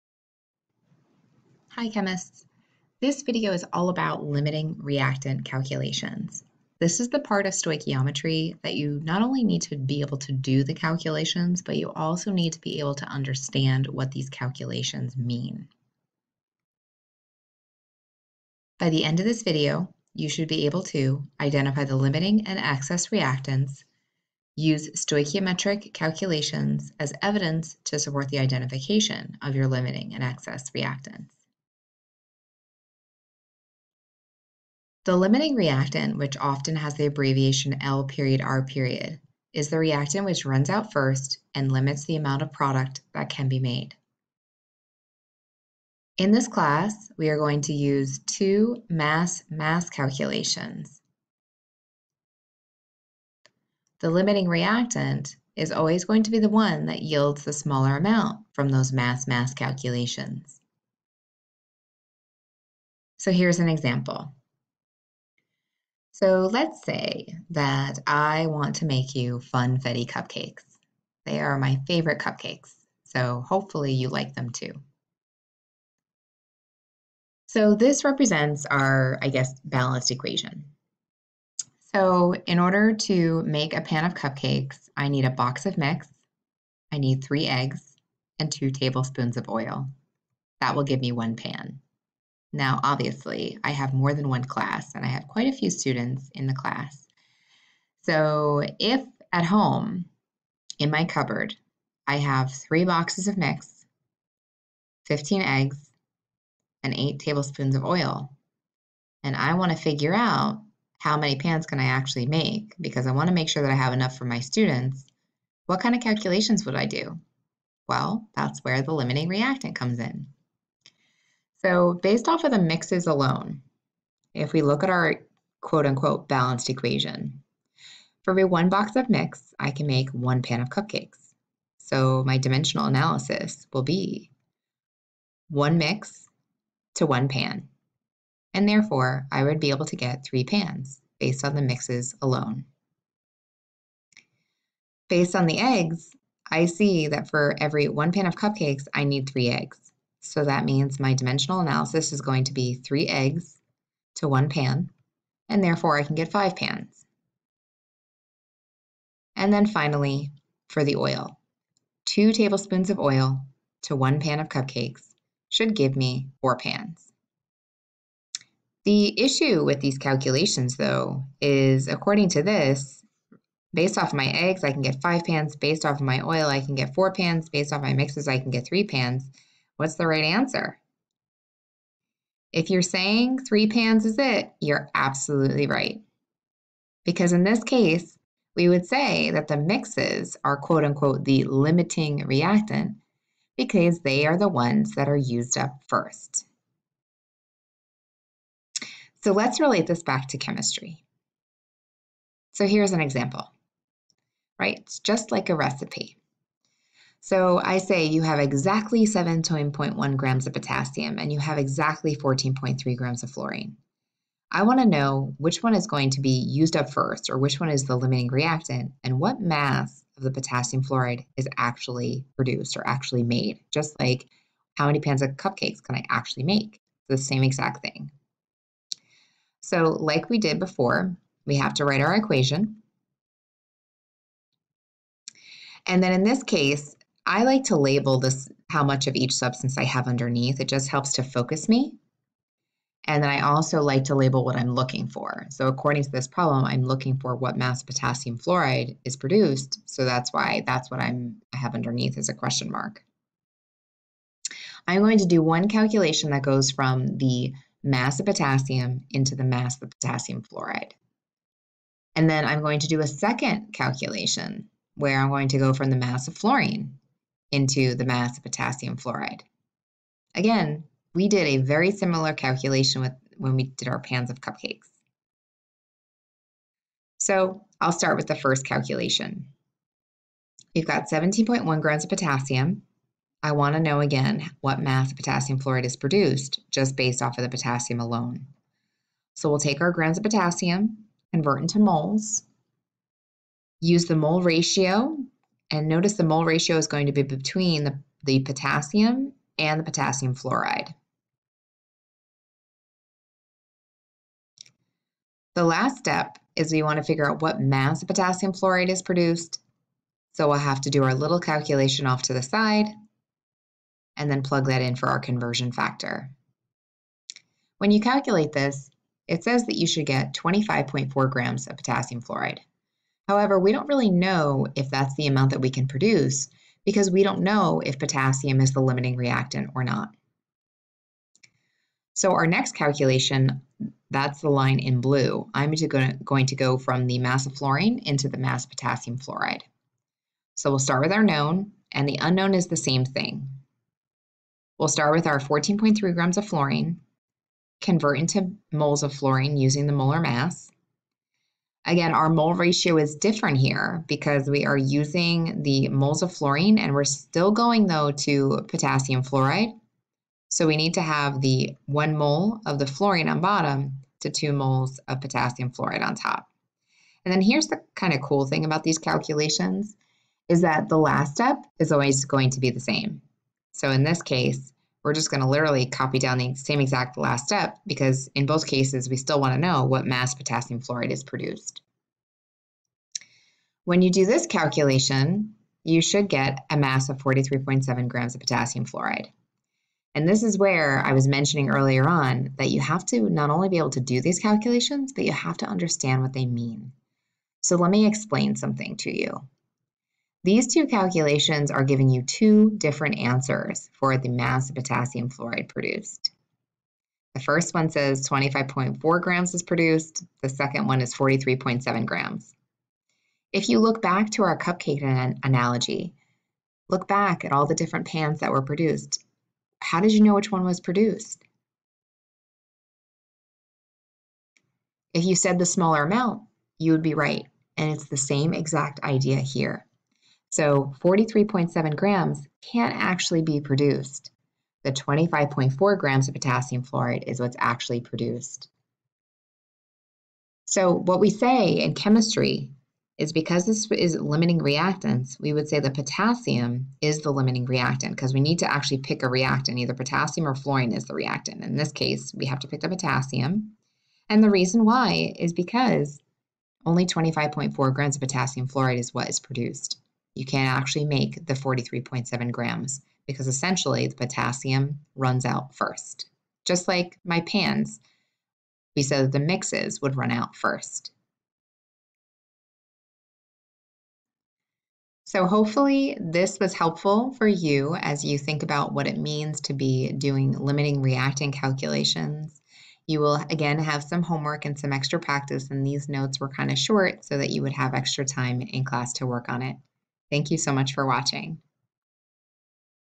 Unit10-Day4-LimitingReacantCalculations
Ms. Razz explains how to perform limiting reactant calculations.